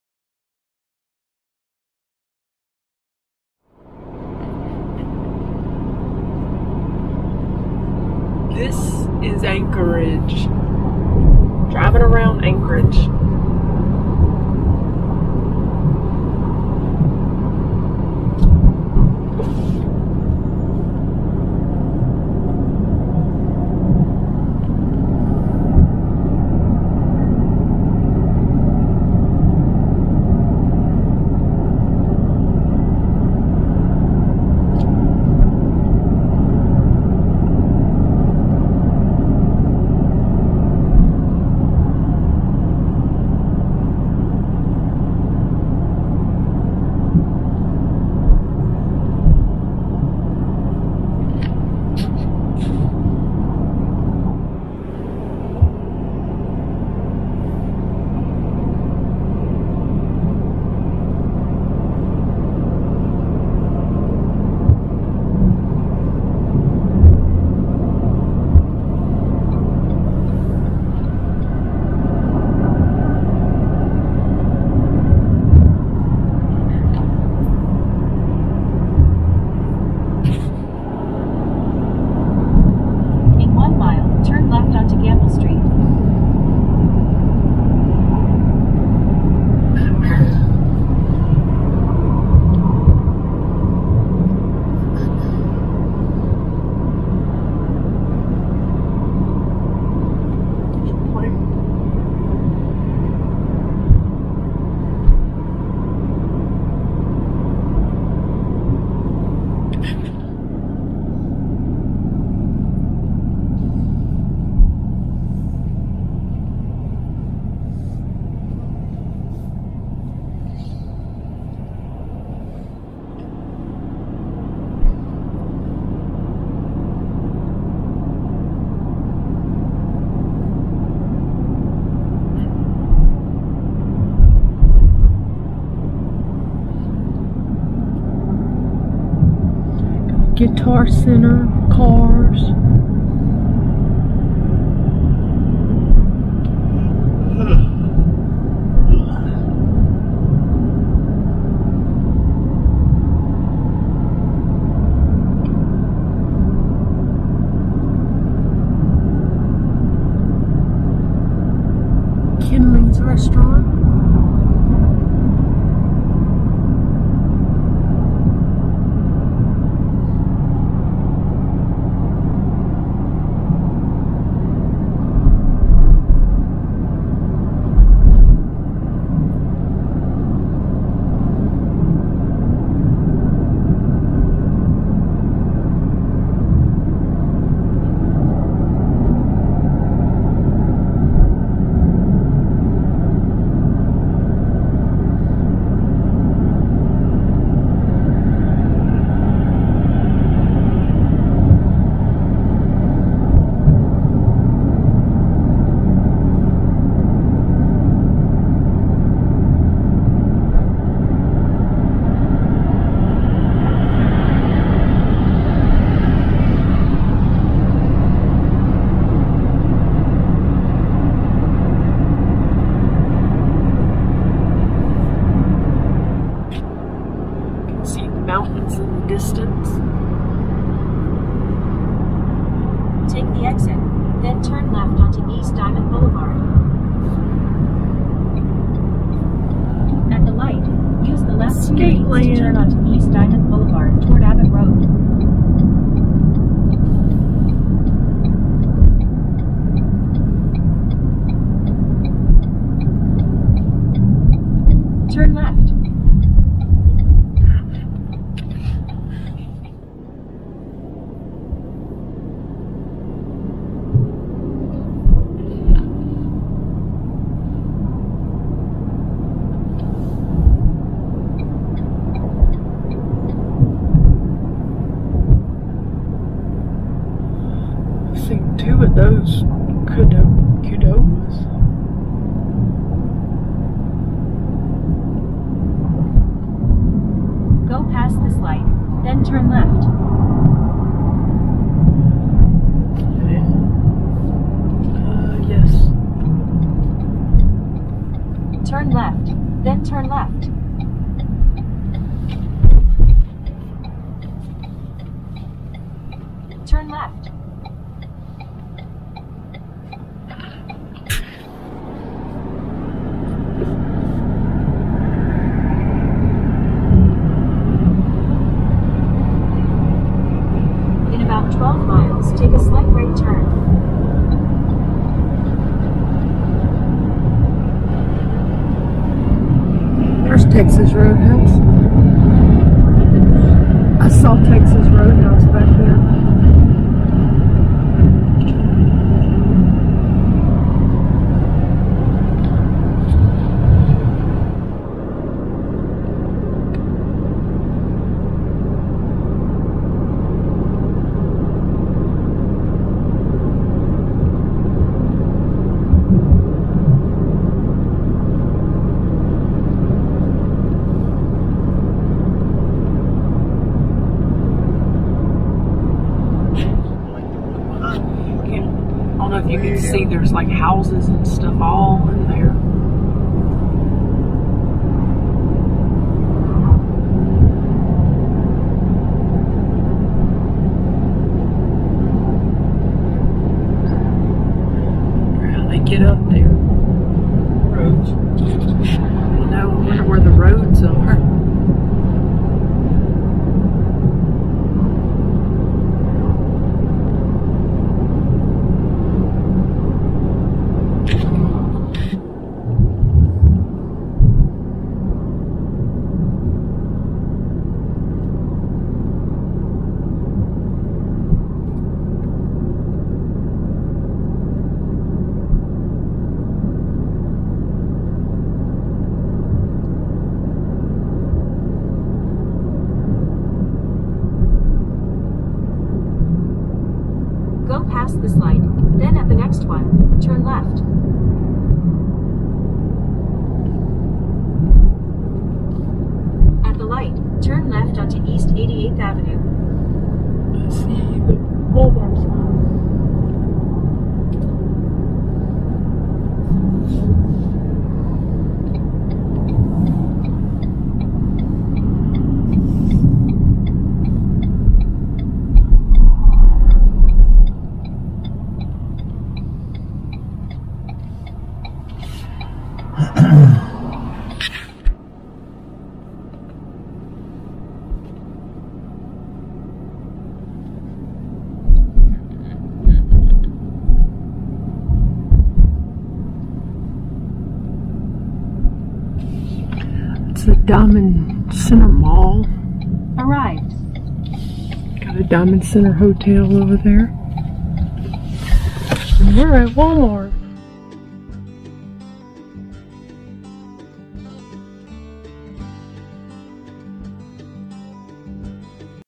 Exploring The Wonders Of Anchorage, Alaska!
Anchorage is the largest city in Alaska and that is where we started. There are many beautiful views in Anchorage and I know we did not get to see them all.